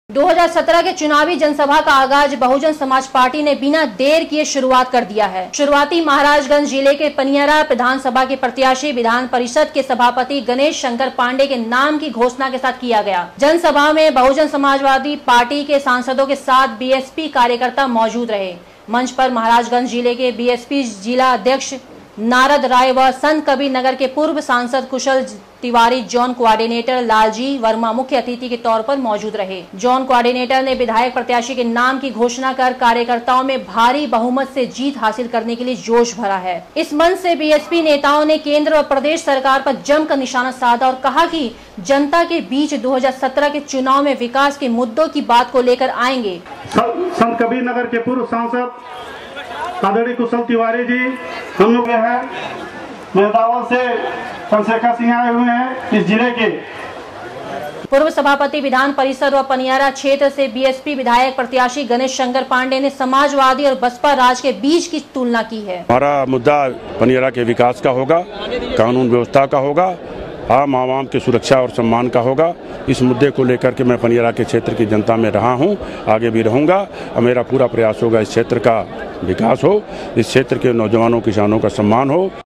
महाराजगंज : बसपा का चुनावी आगाज
2017 के चुनावी जन सभा का आगाज   बीएसपी ने बिना देर किये शुरुआत कर दिया है ,,  शुरुआत महाराजगंज जिले के पनियरा विधान सभा के प्रत्याशी  विधान परिषद के सभापति गणेश शंकर पाण्डेय के नाम की घोषणा के साथ किया गया है ,, जनसभा में बीएसपी के सांसदों के साथ बीएसपी कार्यकर्ता मौजूद रहे ,,मंच पर महराजगंज जिले बीएसपी जिला अध्यक्ष नारद राय व संत कबीर नगर के पूर्व सांसद कुशल तिवारी जोन क्वाडिनेटर लाल जी वर्मा मुख्य अतिथि के तौर पर मौजूद रहे| जोन क्वाडिनेटर ने विधायक प्रत्याशी के नाम की घोषणा कर कार्यकर्ताओ में भारी बहुमत से जीत हासिल करने के लिए जोश भरा है इस चुनावी जन सभा में पूरे  पनियरा टाउन को  नीले रंग में झंडे से सजाया गया था ,,इस मंच से बीएसपी नेताओ ने केन्द्र व प्रदेश सरकार पर जमकर निशाना साधा है और कहा की जनता के बीच  2017 के  चुनाव में विकास के मुद्दो की बात को लेकर जाएंगे
पूर्व सभापति विधान परिषद व पनियरा क्षेत्र से बीएसपी विधायक प्रत्याशी  गणेश शंकर पाण्डेय ने समाज वादी और बसपा राज के बीच की तुलना की है --